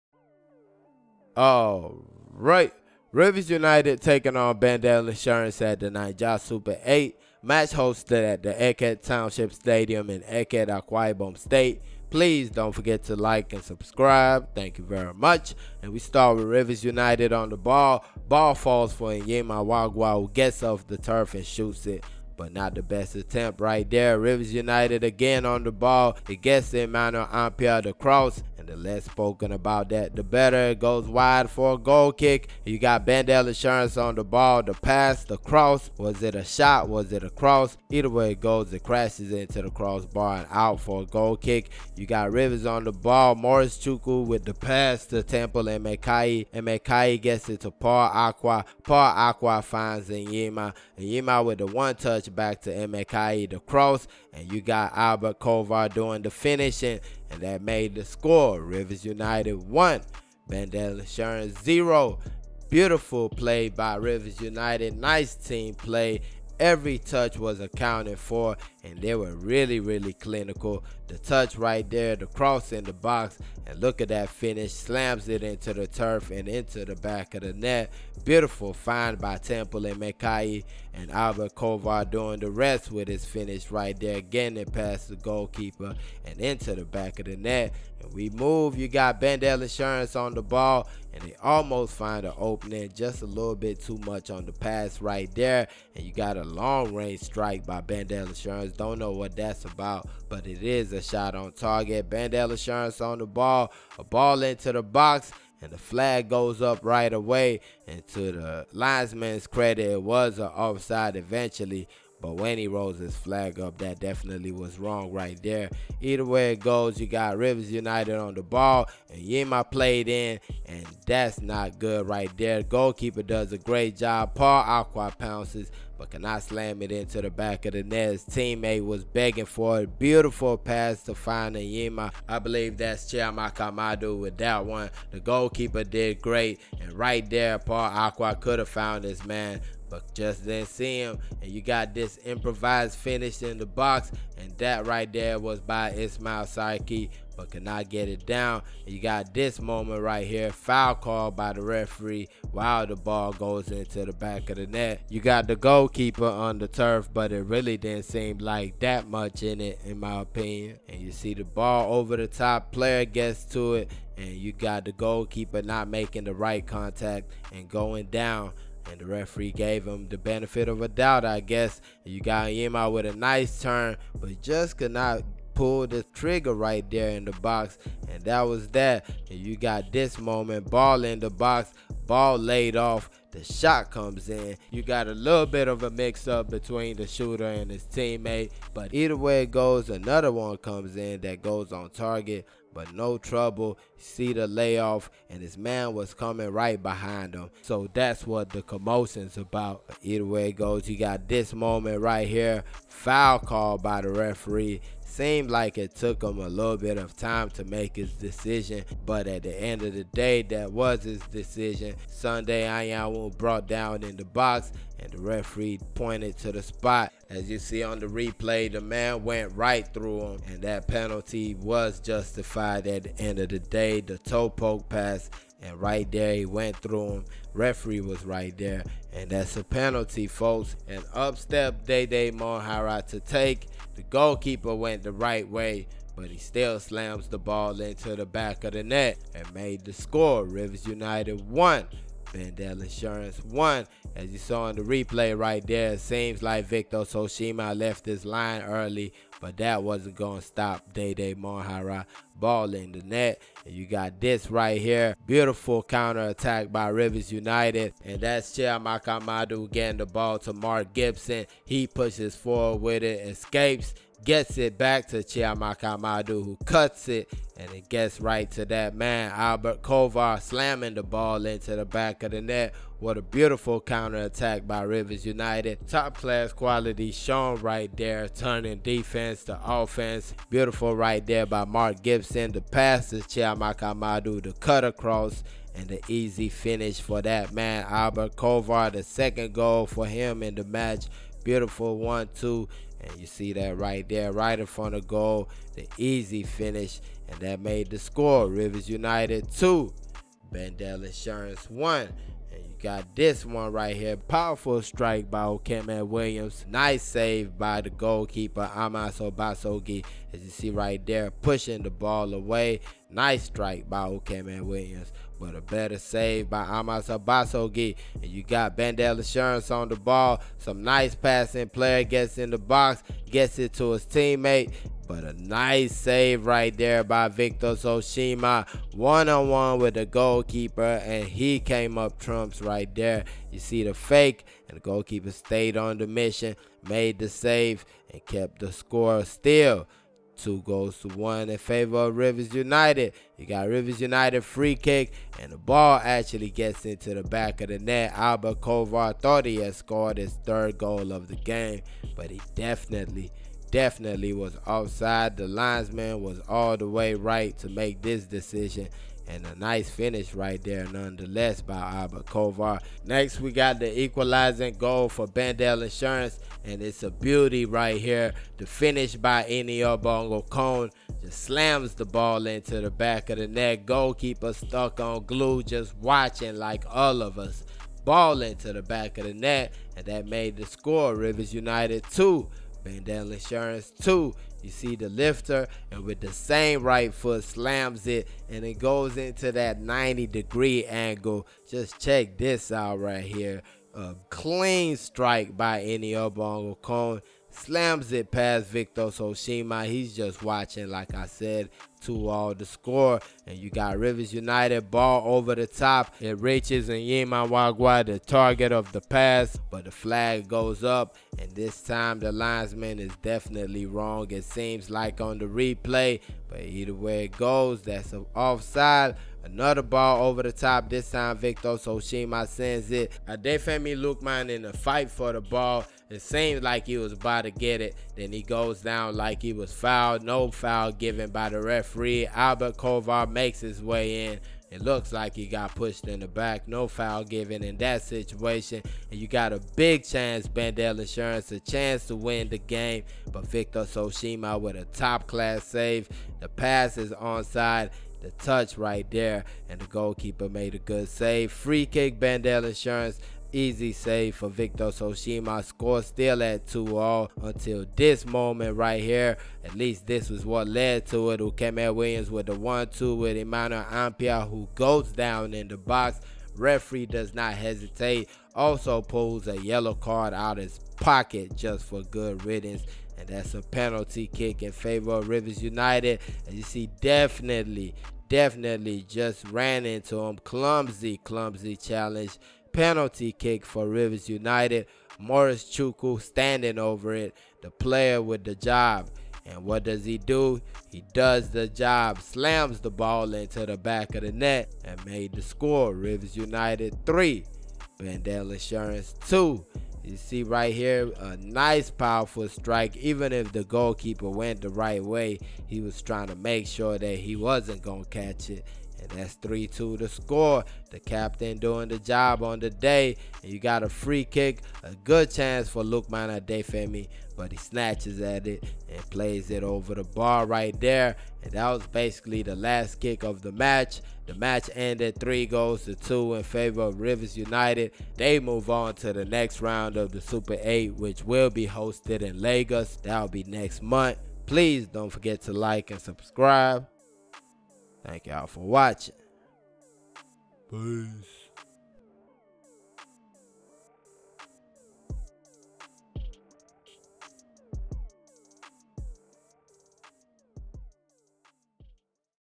Rivers United vs Bendel Insurance [3-2] Wonder Goal @ Naija Super 8
Naija Super 8 action as Rivers United defeat Bendel Insurance in an instant classic. Both Rivers United and Bendel Insurance scored top class goals in what was the best match so far in the Naija Super 8 tournament.